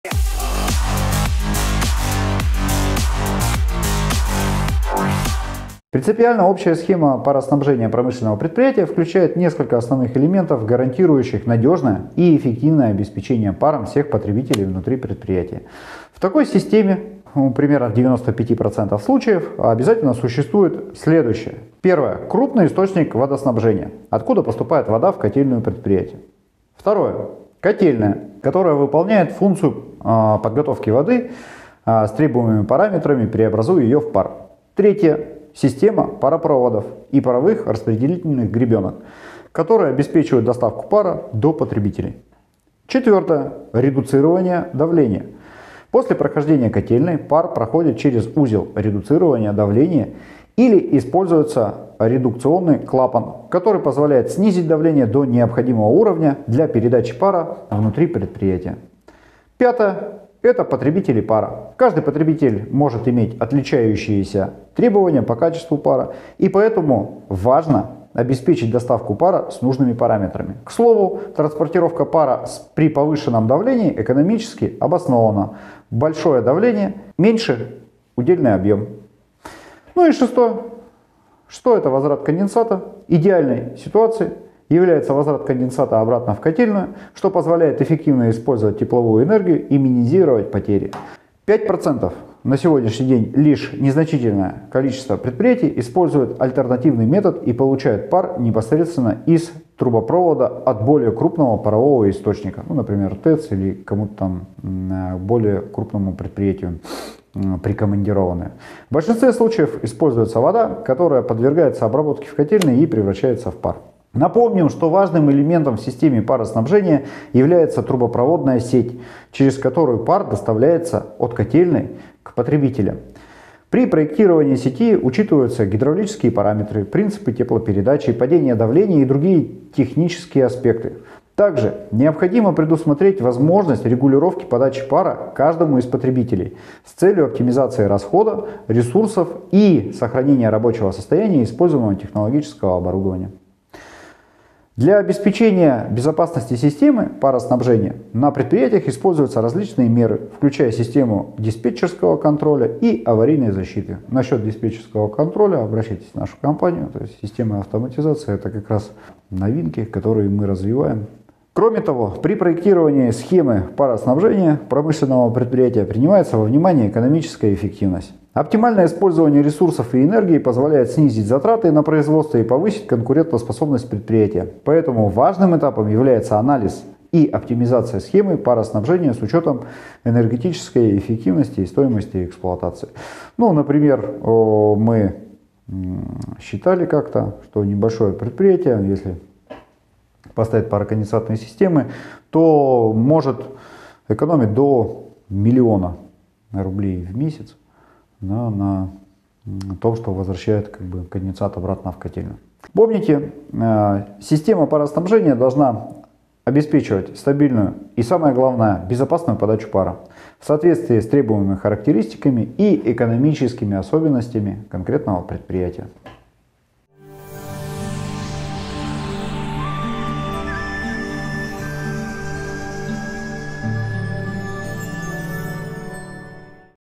Энергоснабжение в действии: пароснабжение промышленных предприятий 🏭⚙️
🏭⚙️ Узнайте о принципах эффективного пароснабжения промышленных предприятий!

Наш экспертный видеоролик расскажет о ключевых элементах общей схемы пароснабжения, включая котельные, трубопроводы, паровые турбины и системы конденсации.

Мы рассмотрим, как эти компоненты работают вместе для обеспечения надежного и эффективного энергоснабжения. 💡🏗️